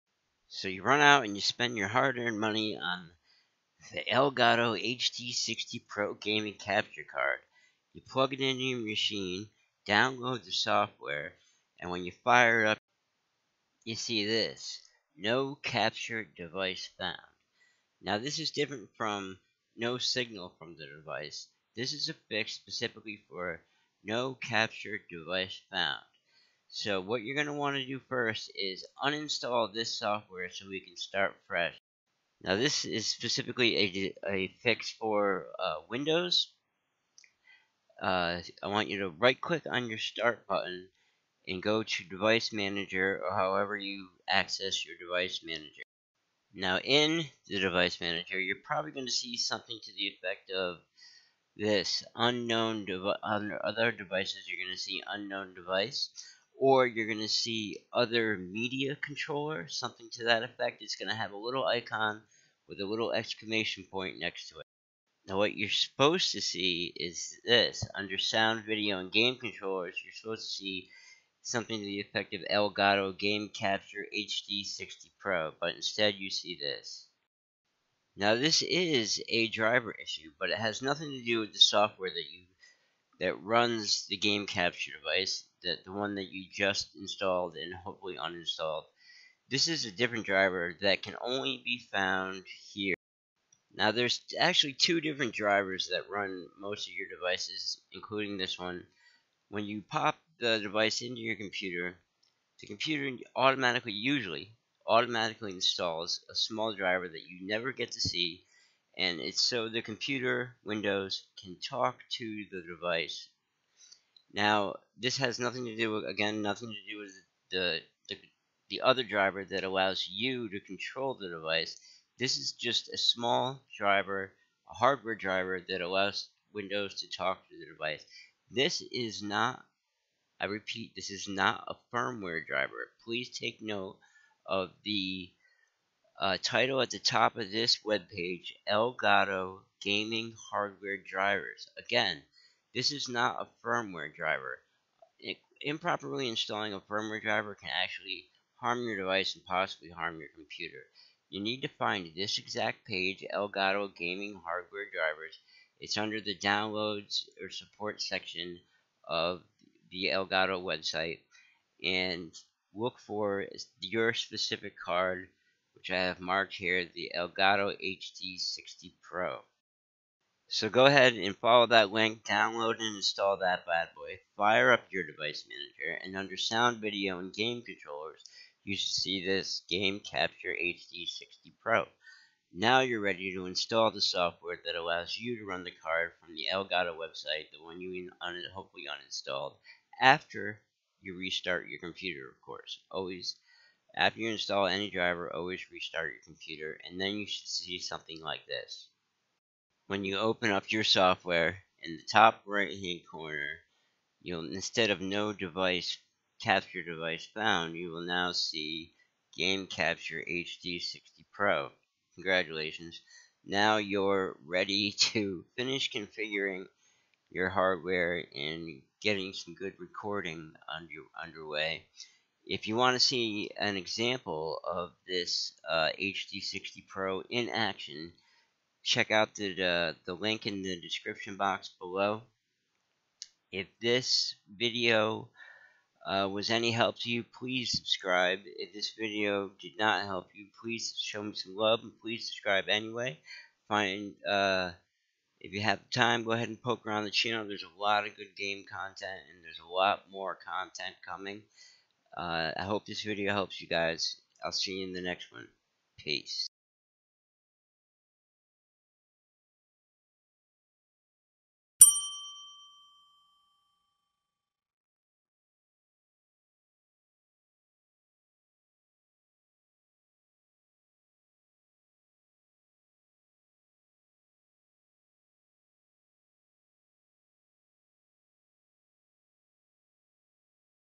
ElGATO HD60 PRO - No Capture Device Found - FIX
A FIX FOR THE " NO CAPTURE DEVICE FOUND " ERROR WHILE INSTALLING THE HD60 PRO.